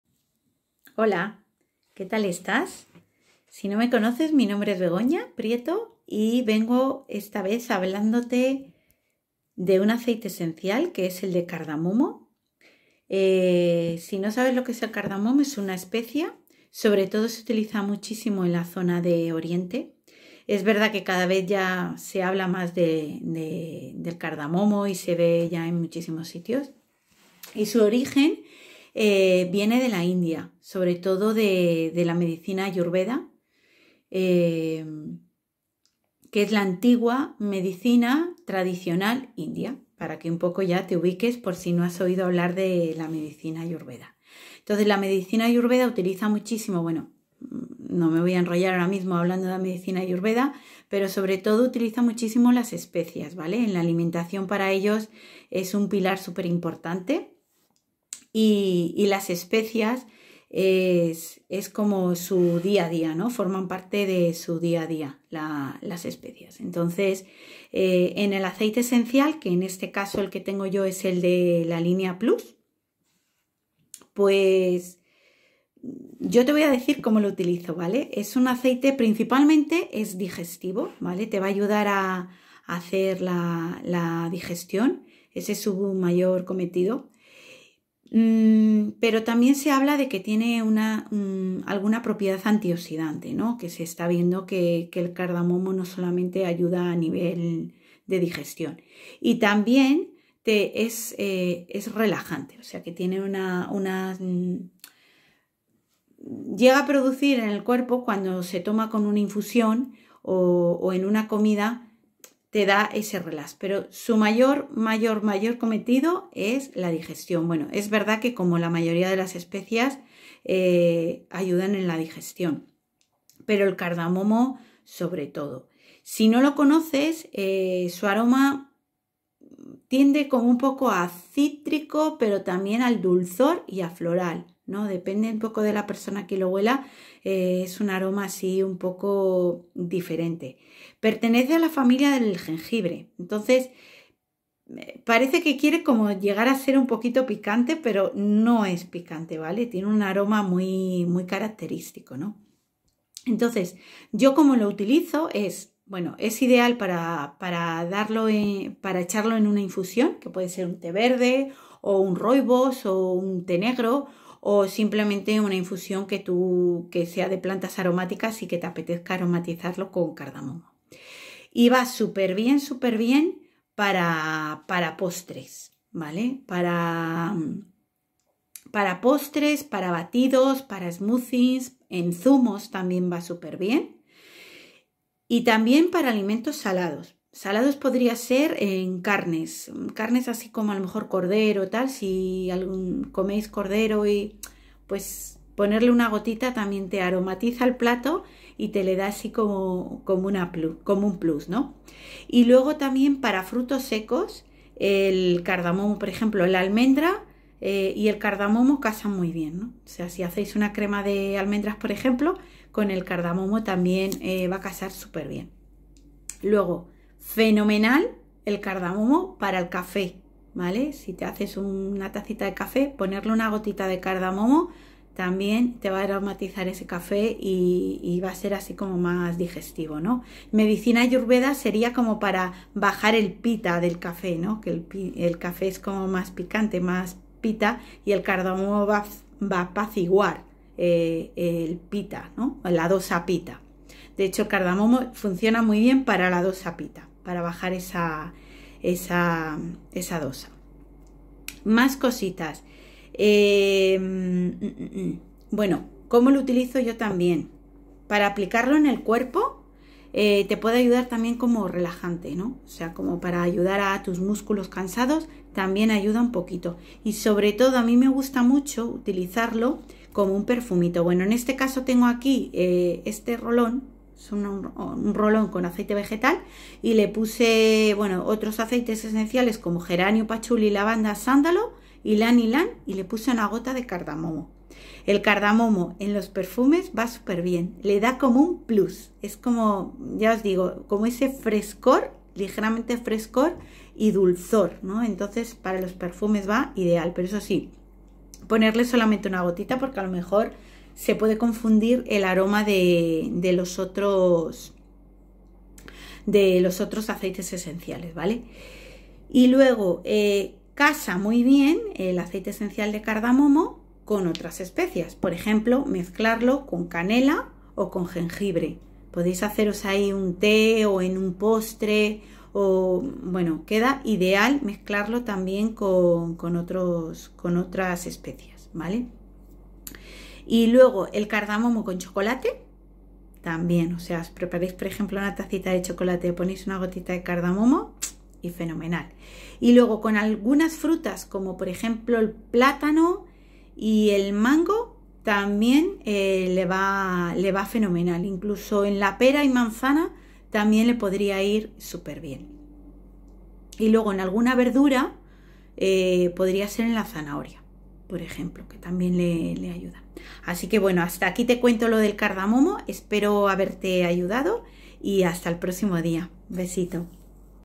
CARDAMOMO de Young Living
Cardamon libera emociones de inseguridad sin importancia que resultan del terror o la agitación emocional y el estrés que se mantiene en el estómago. Esto normalmente se asocia con emociones no procesadas.

Video creado por integrantes de Comunidad Mensajeros Aromáticos con Young Living. Gracias Begoña Prieto por el aporte.